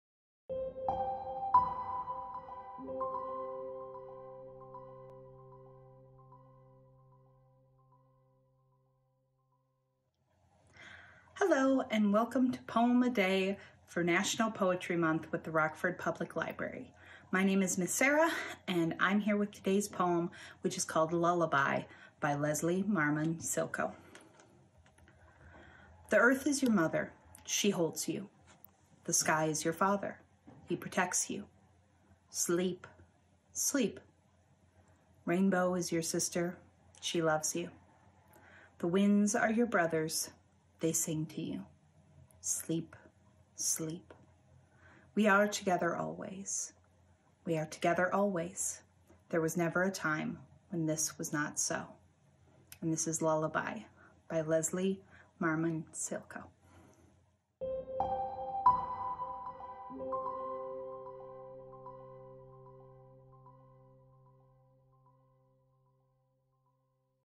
Lullaby - A Poem a Day | April 29, 2020
Today's Poem a Day selection is Lullaby by Leslie Marmon Silko read by Ms. Sarah!